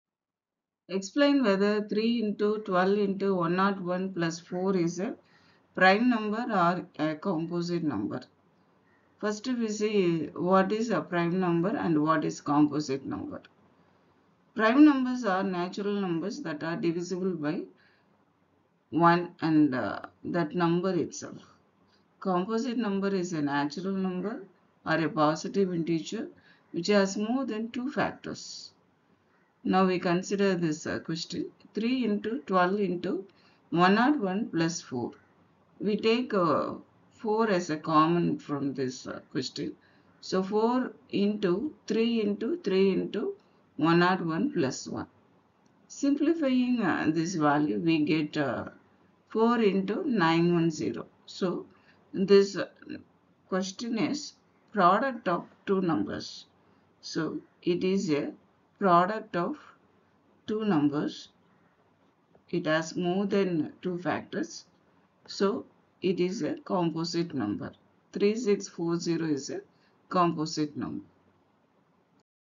Maths /NCERT/Real Numbers/Assignment Question/ Explain whether 3× 12 × 101 + 4 is a prime number
Maths /NCERT/Real Numbers/Assignment Question/ Explain whether 3× 12 × 101 + 4 is a prime number or a composite number#10maths